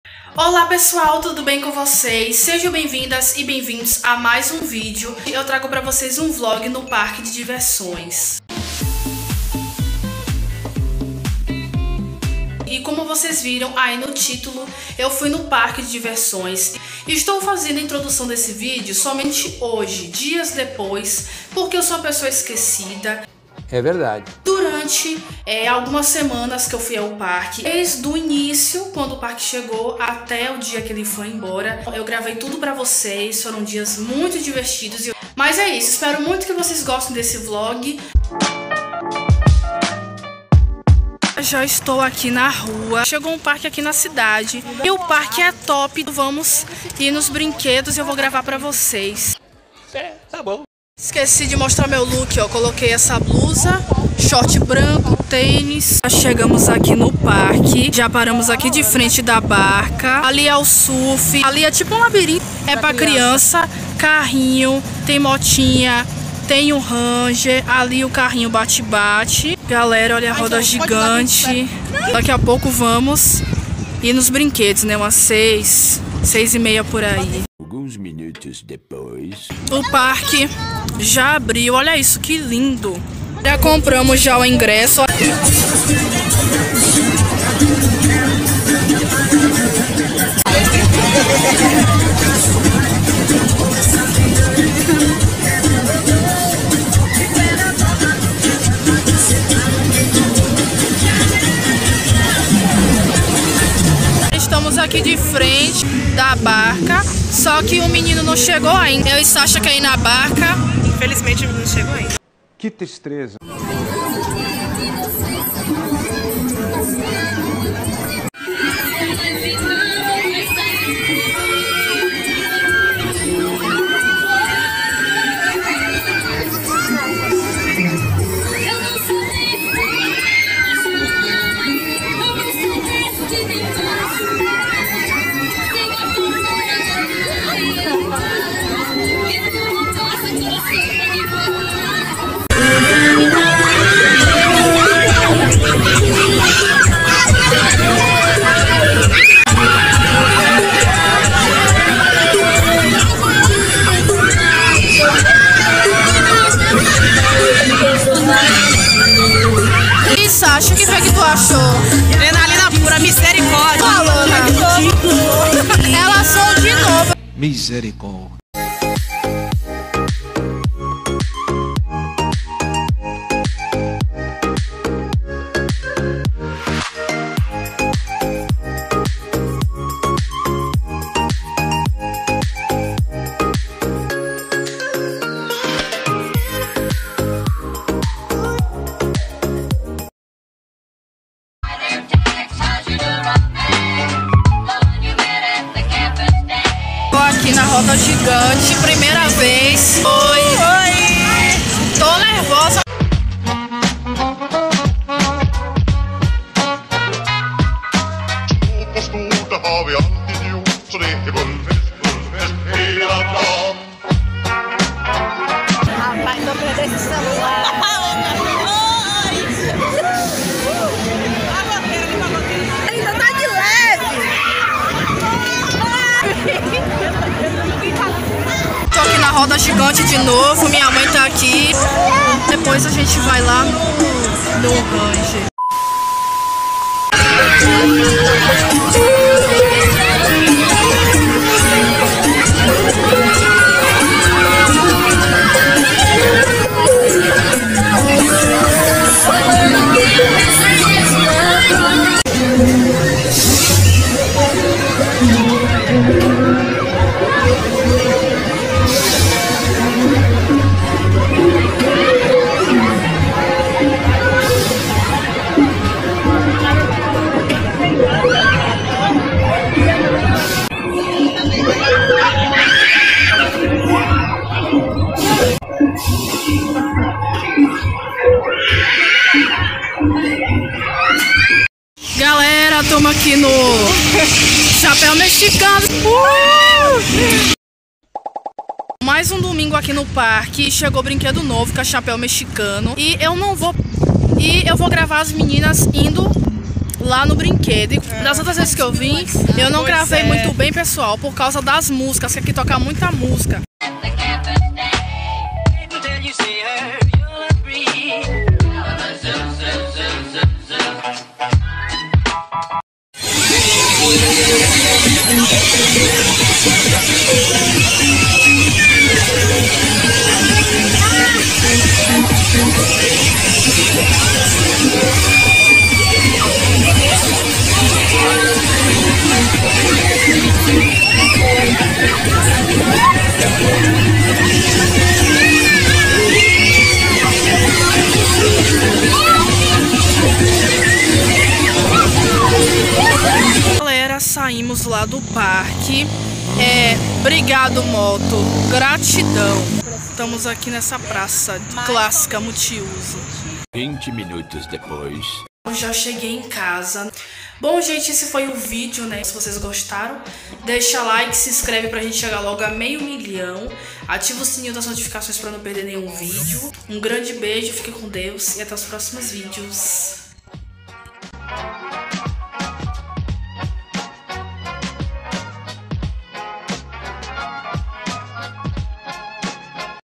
VLOG NO PARQUE DE DIVERSÕES 🎡
FUI AO PARQUE DE DIVERSÕES E FILMEI TUDO PRA VOCÊS ESPERO QUE GOSTEM FIQUEM COM DEUS ❤️